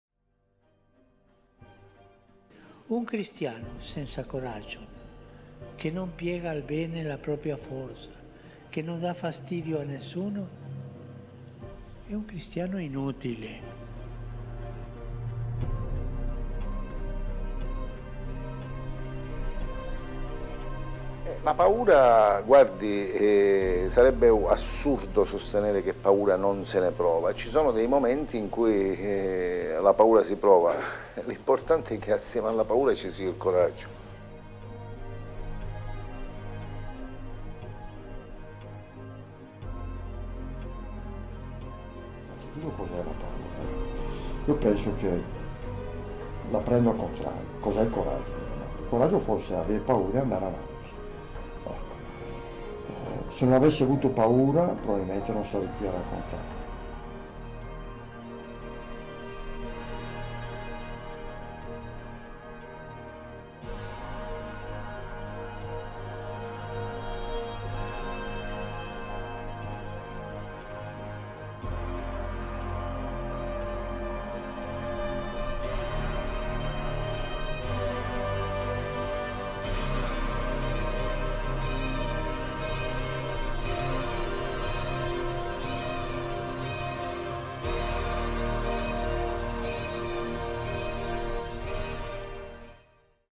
Vai Avanti !
citazioni di Papa Francesco, Paolo Borsellino, Roberto Ghidoni, e Maria Teresa di Calcutta -
montaggio Lorenzo Amaro